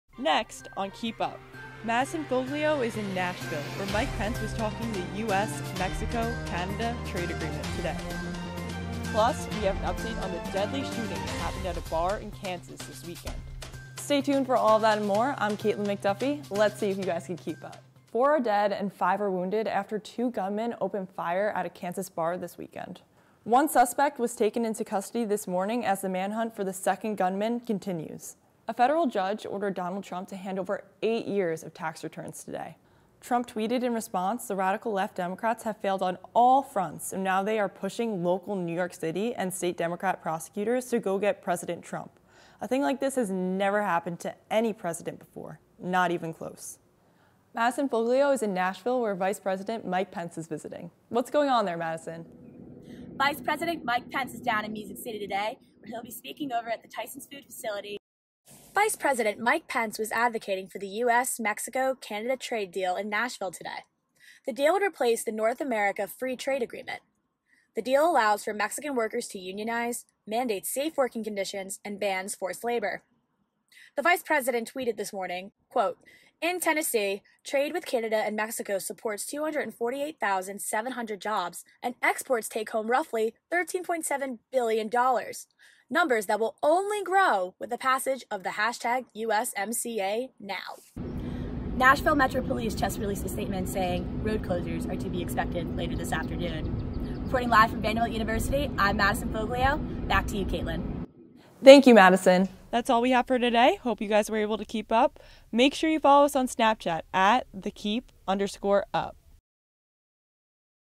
The Keep Up: 10/7/19
VP Mike Pence vists Nashville, TN. Vanderbilt University correspondent Madison Foglio is on the scene.